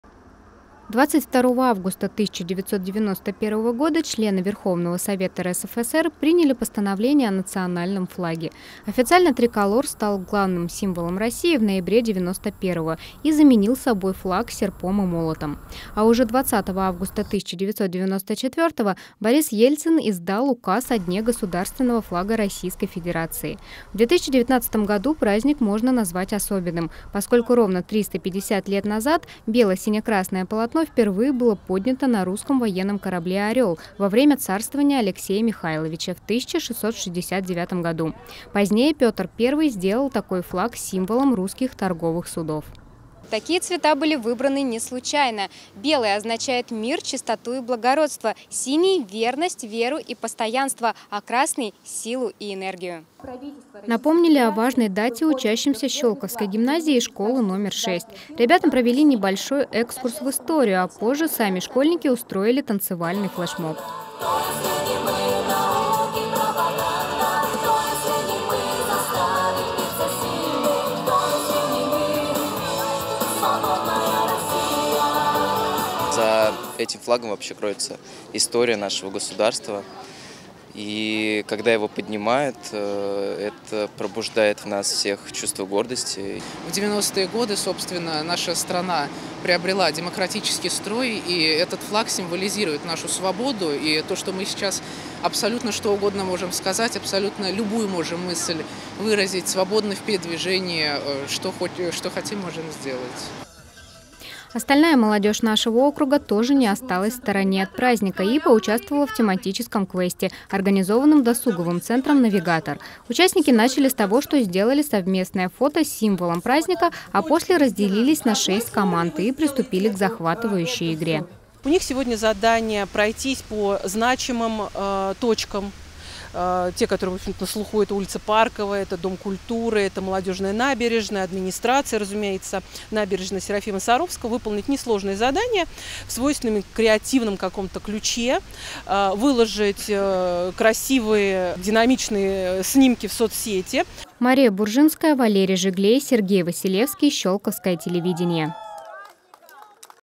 День российского флага
Ежегодно 22-го августа в России отмечается День государственного флага. Вспомнили о знаменательной для страны дате и в нашем городском округе.